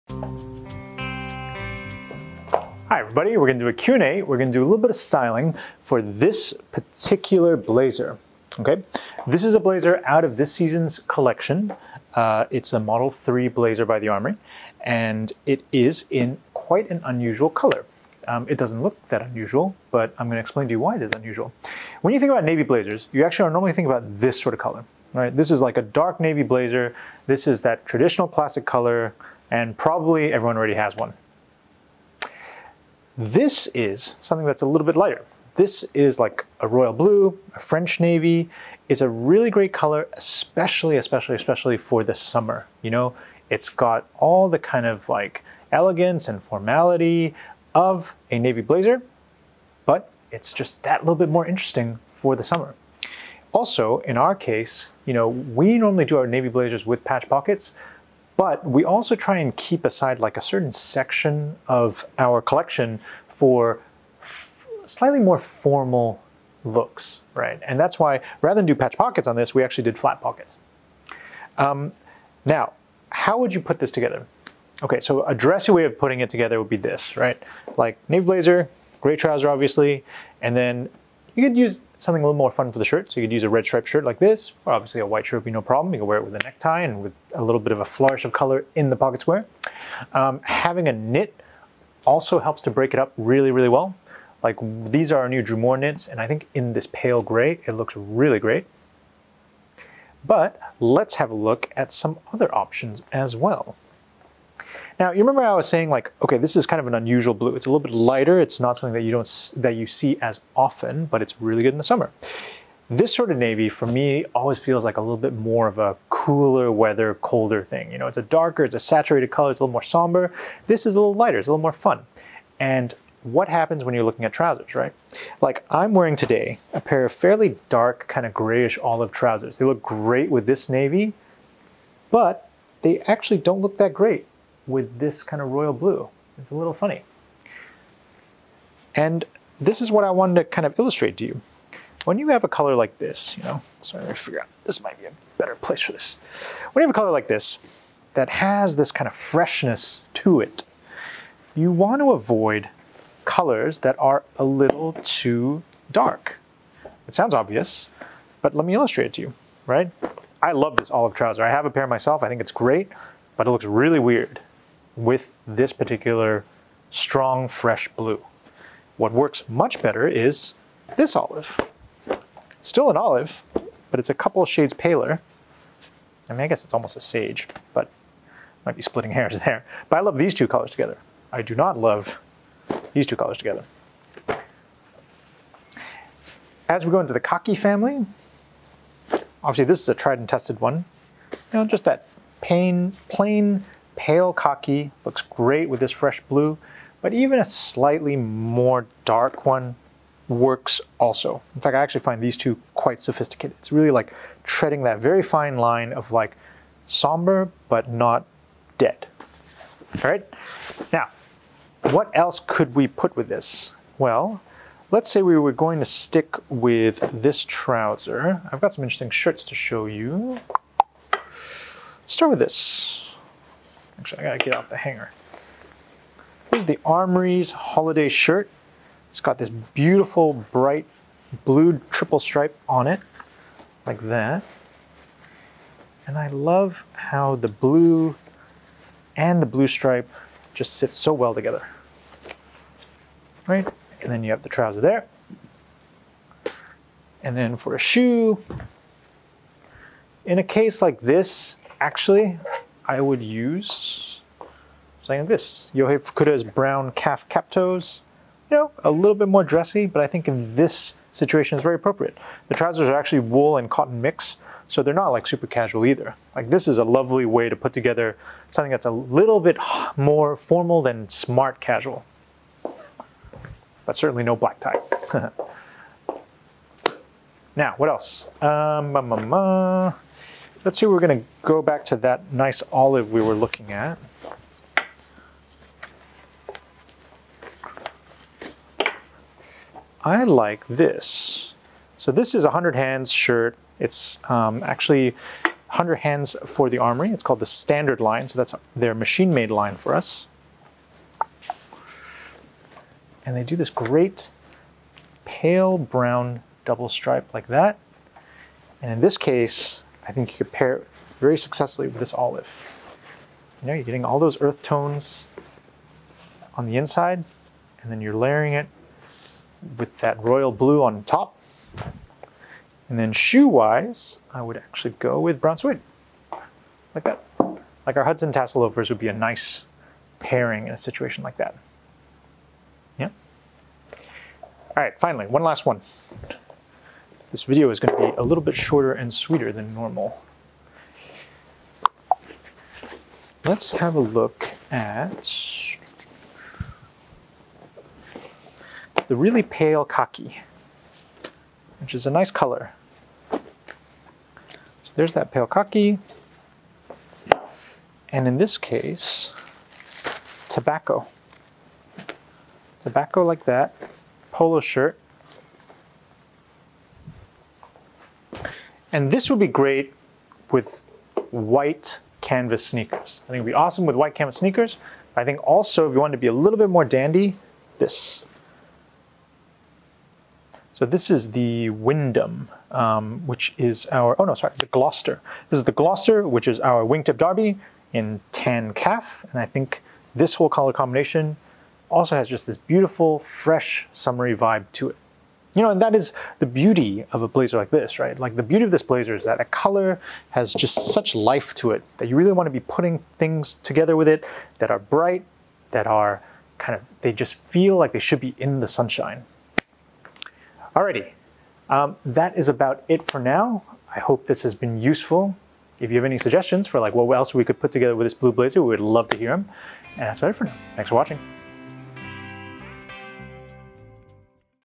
How to Style a Royal Blue blazer - Q&A
Our series of Questions and Answers with The Armoury Founders, Mark Cho and Alan See. In this video we take a closer look at the subtle difference between royal blue and navy blue sport coats.